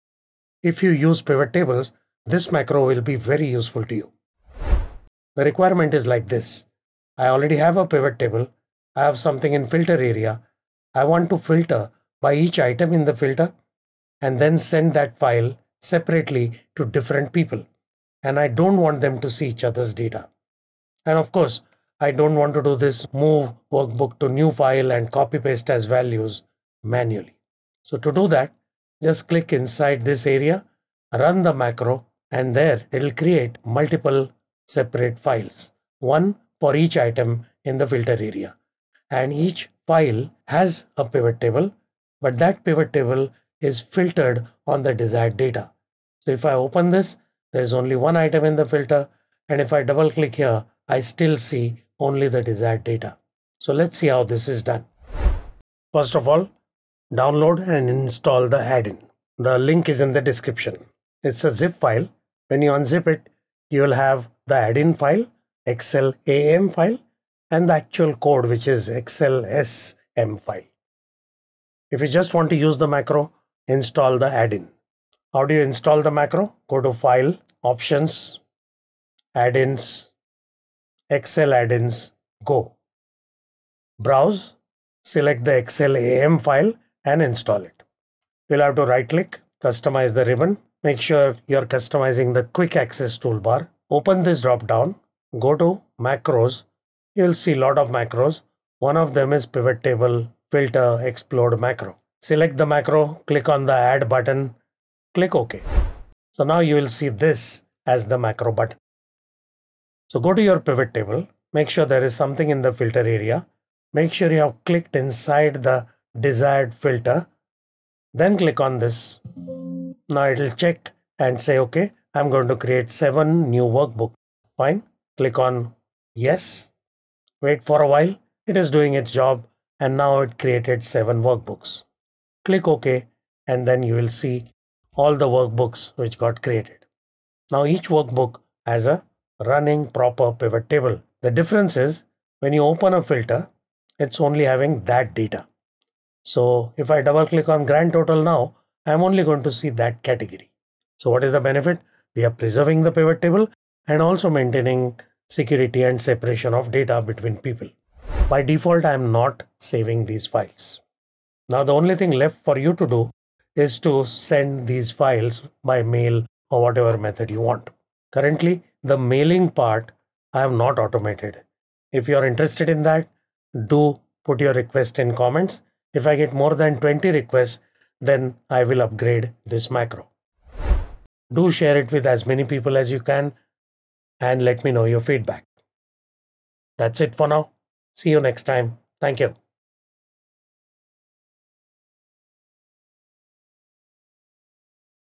Pivot Filter Split Macro - Download VBA code
Do you filter pivot tables one item at a time and copy paste and send to different people? Stop doing that. Use this macro by Dr Nitin.
It saves you all the trouble and gives you readymade pivot files.
You just have to mail them. Pivot table is preserved. No need to paste as values. Each pivot has only the filtered data. So security is also maintained.

Efficiency Best Practices for Microsoft 365
130 best practices everyone must know